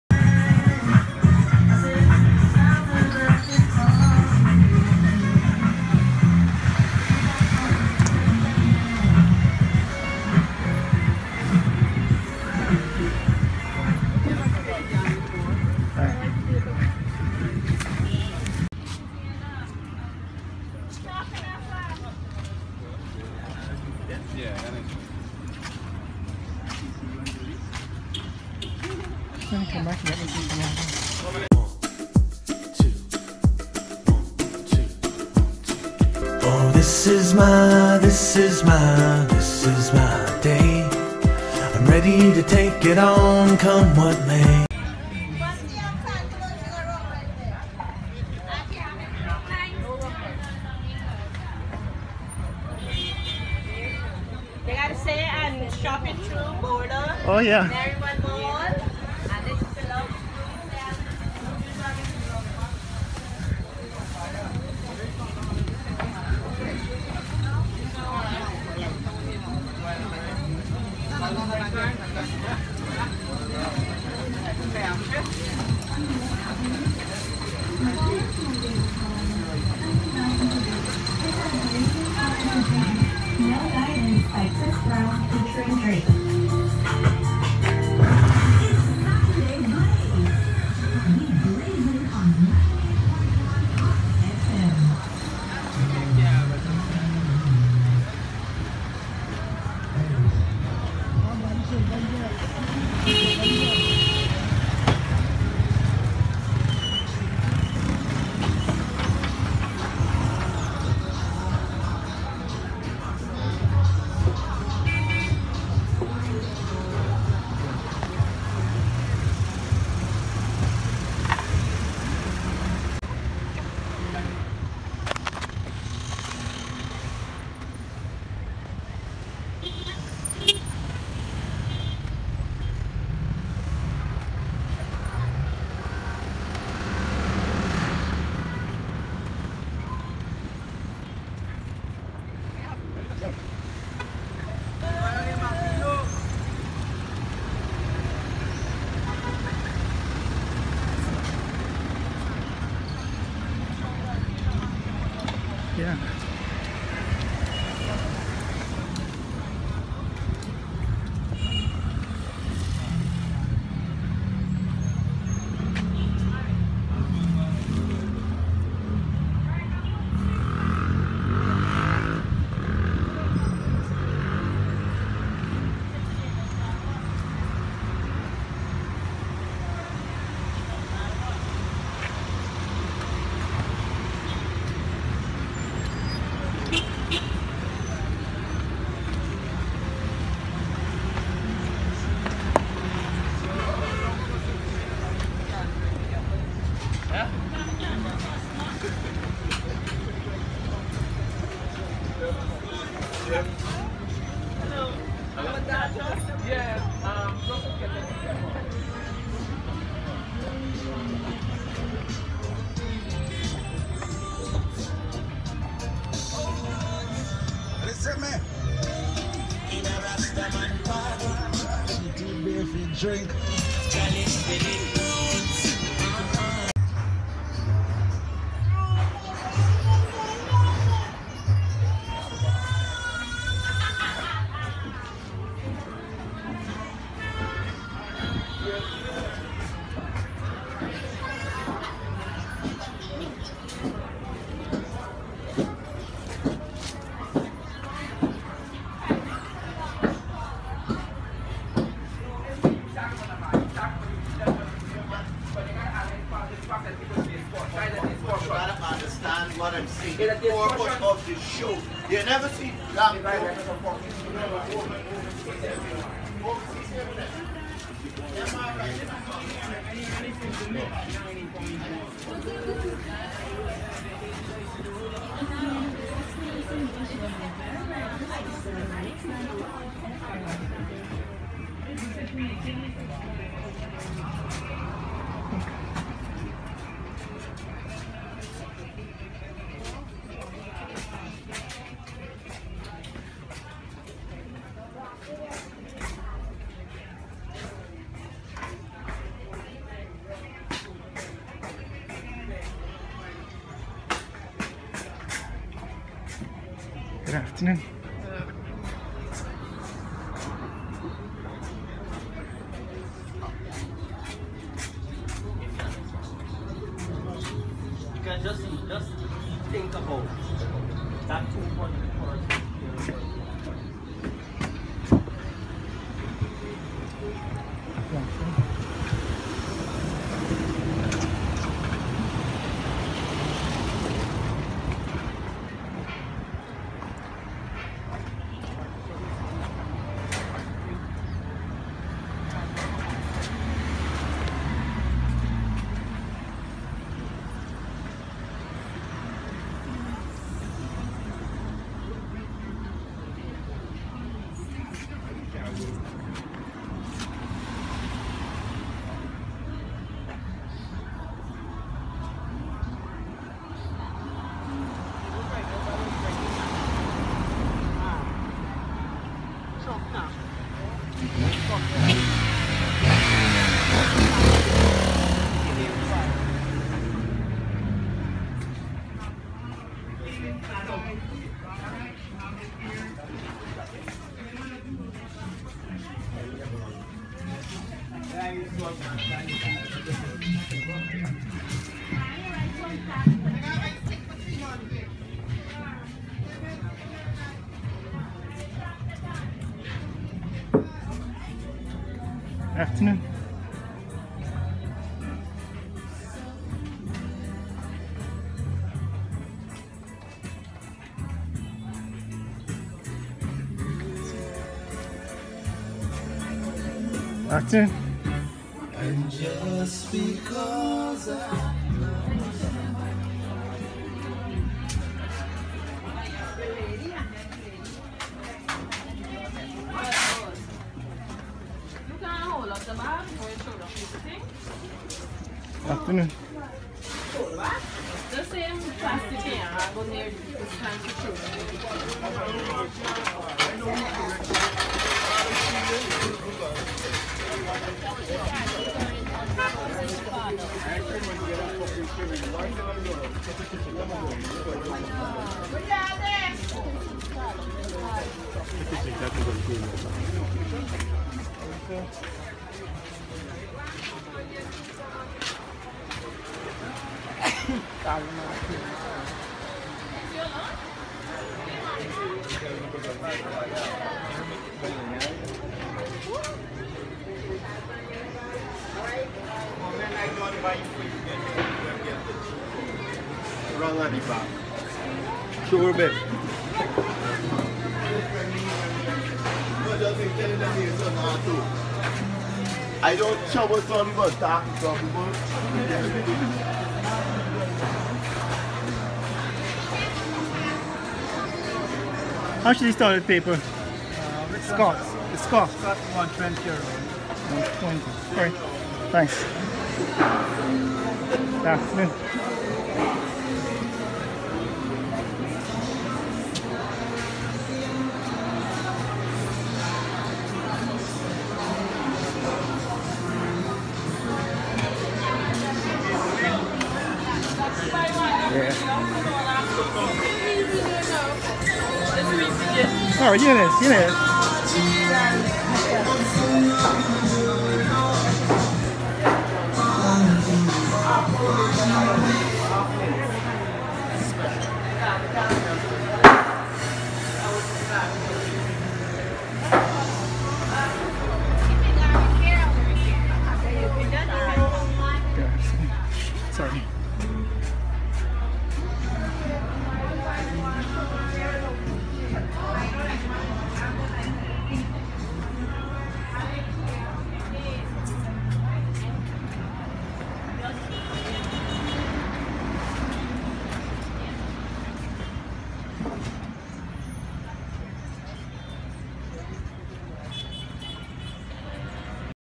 S7.E6 | Walk with me Through Bourda Green and Bourda Market
Walk with me on Bourda Green and through Bourda Market on a rainy day in November 23 2019.